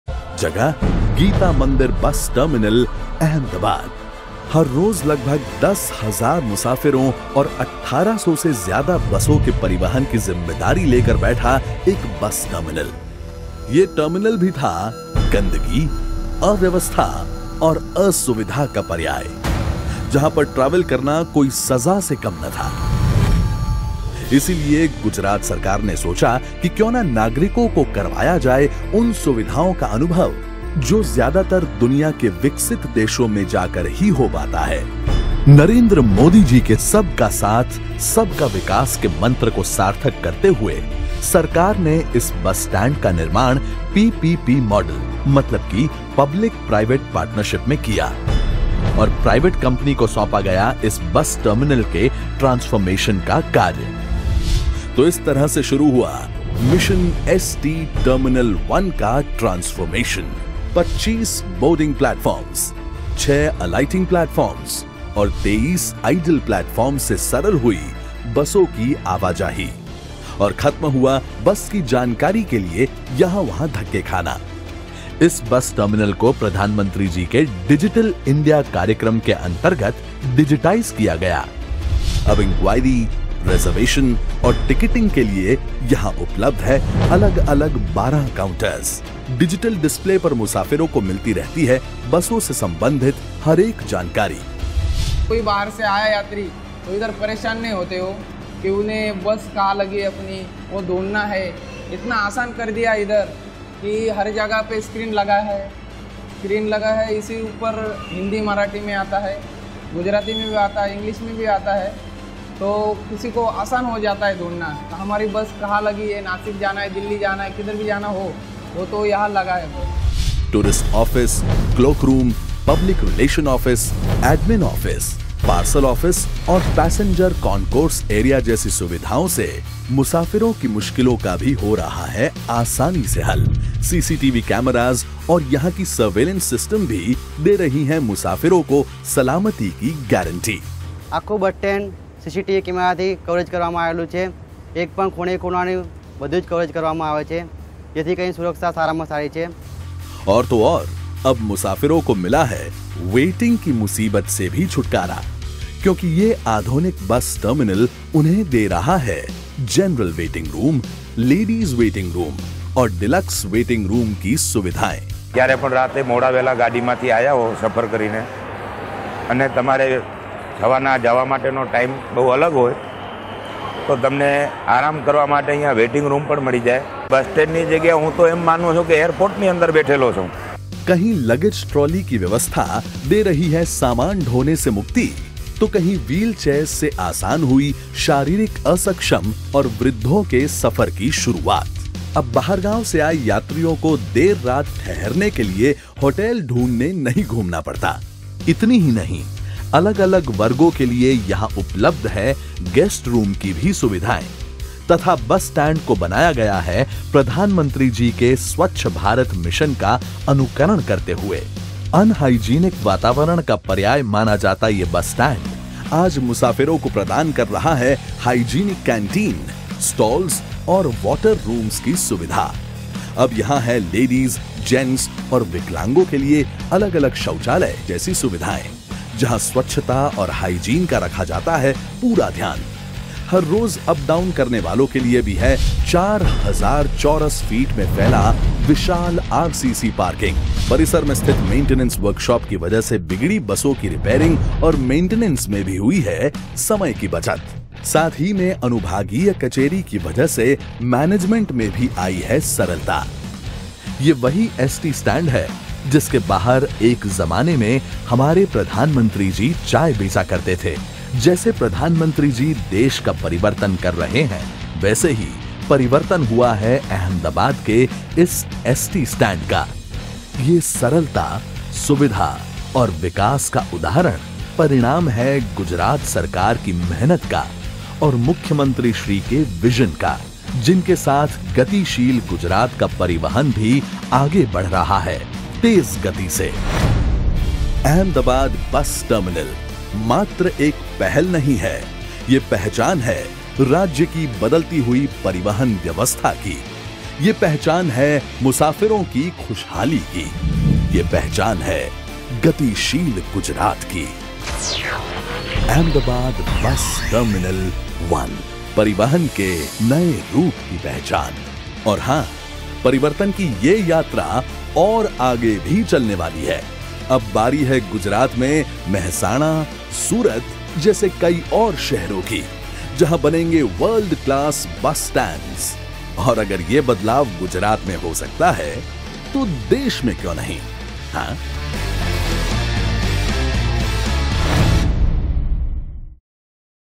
GSRTC Bus terminal, Gita Mandir, Ahmedabad
A look at the GSRTC Bus terminal at Ahmedabad, indeed a fine example of PM Narendra Modi's vision of Sabka Sath Sabka Vikas